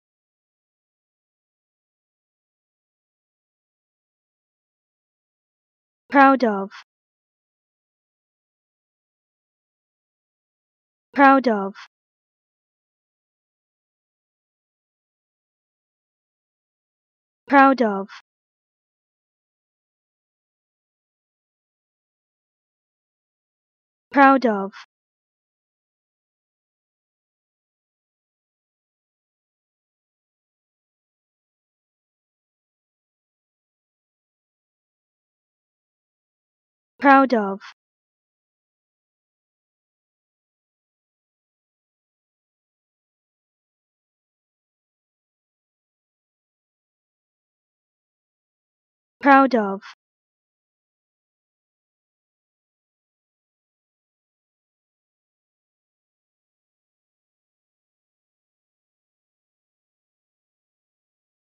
How To Say Proud Of
Learn how to say Proud Of with EmmaSaying free pronunciation tutorials.